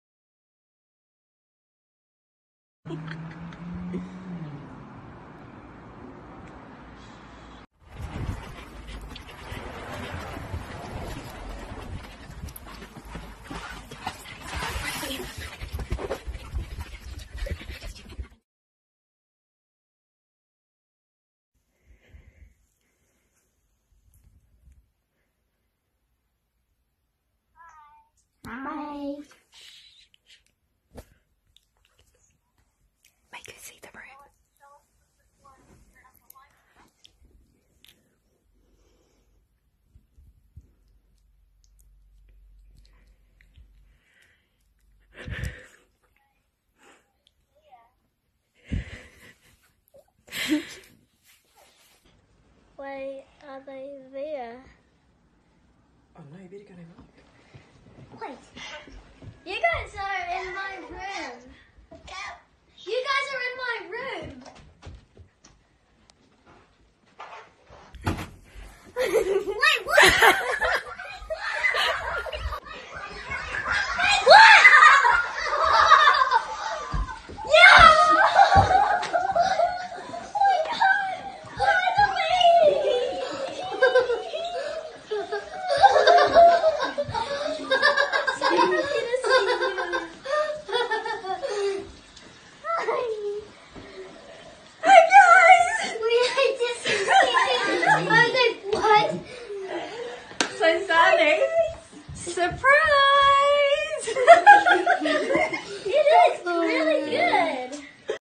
Cousins reunion after lockdown
Zarnie and Evanee were surprised by their cousins, who drove 3 hours to visit after New Zealand’s Alert Level 4 lockdown.

Here is our edited video from both perspectives - it was a wonderful moment to be a part of :)